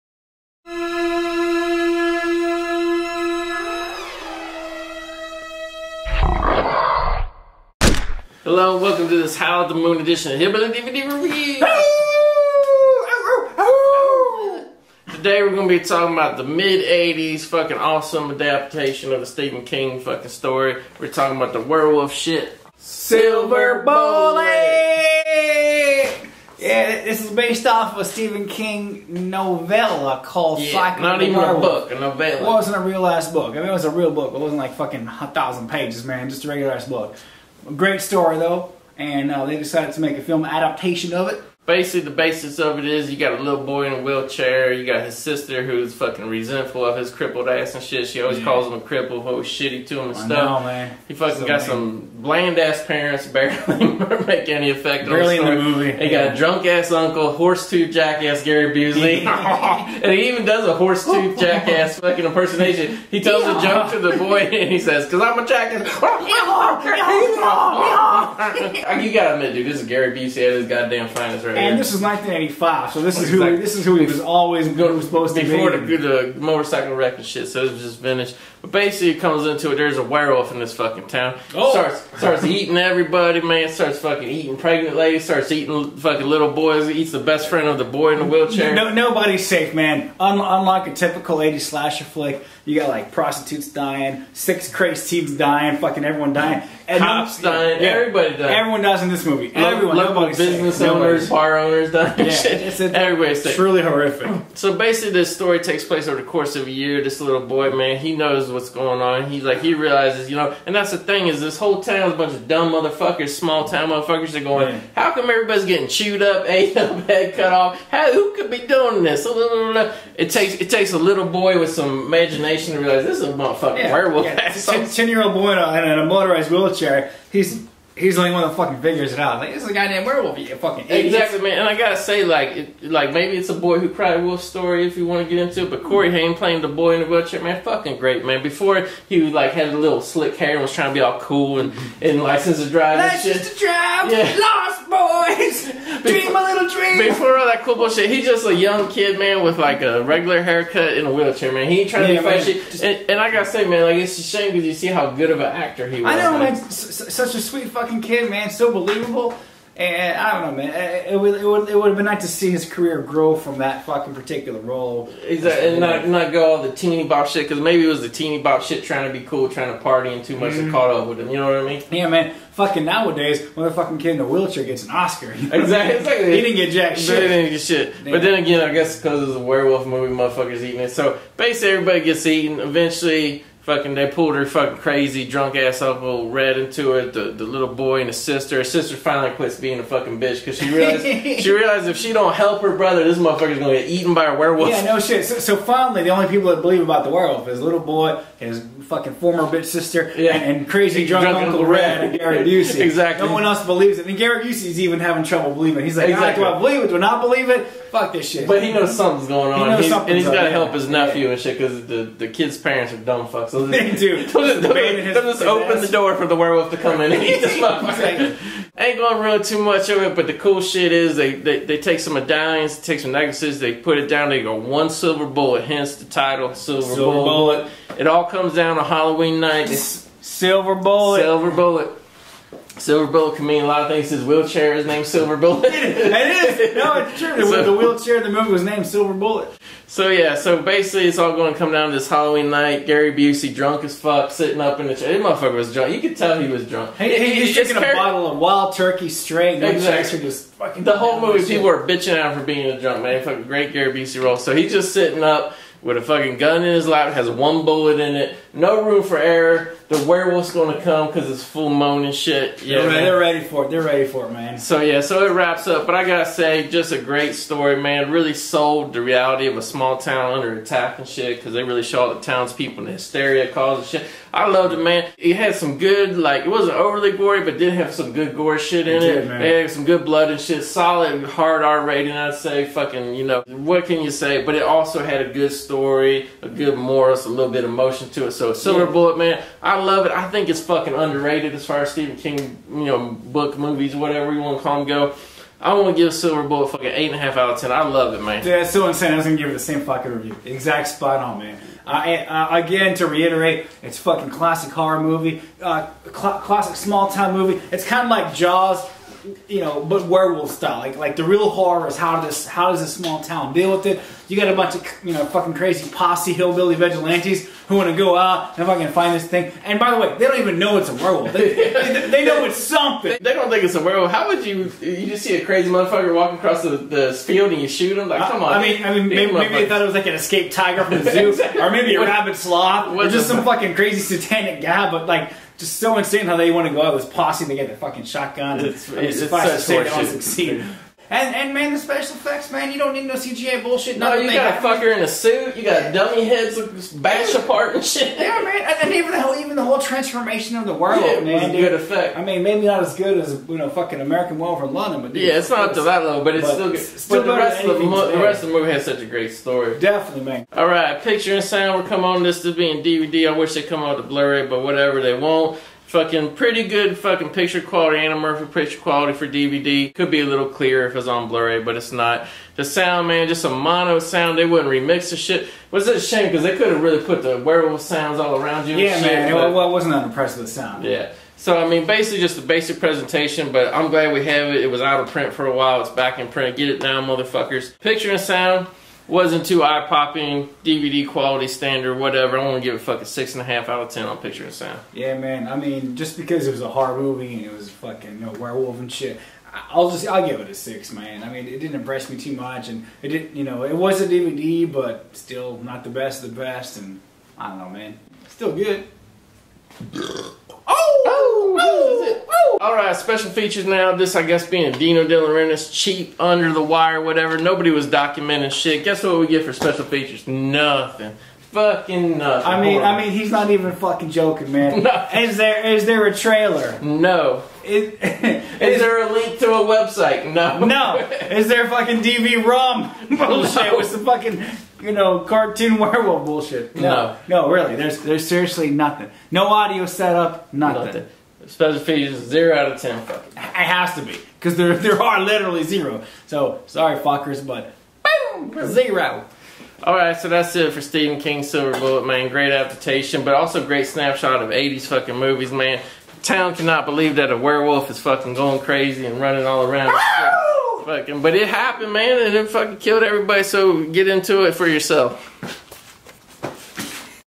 SILVER BULLET dvd review
Phil Dease & The Goat bark at the moon with the dvd review of Silver Bullet (1985) starring Corey Haim, Gary Busey, Everett McGill, Megan Follows and directed by Daniel Attias.

Have you seen this movie? Could your drunk uncle kill a 8 foot tall werewolf? What did you think of it? Let us know by leaving a comment in the comments section or uploading a video response!